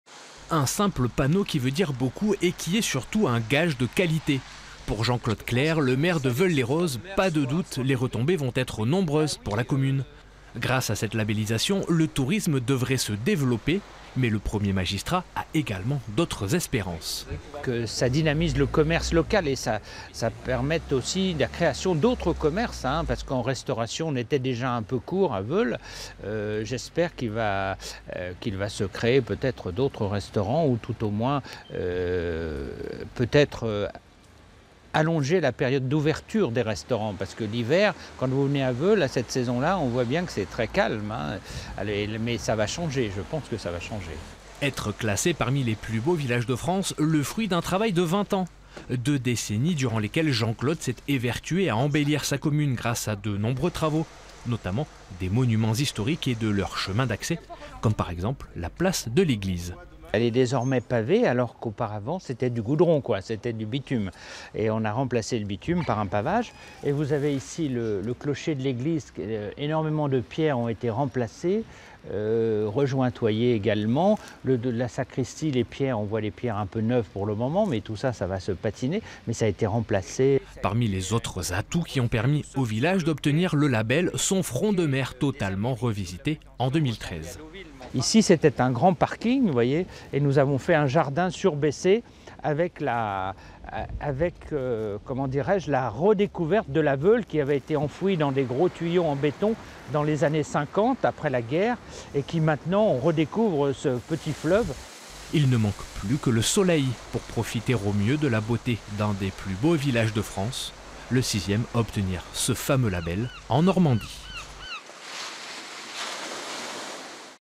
Veules-les-Roses labellisé « Plus beaux villages de France »
Le joli village de Veules-les-Roses vient d’être labellisé « Plus beaux villages de France ». Une belle récompense pour cette commune de Seine-Maritime qui a entrepris de gros efforts pour recevoir cette récompense.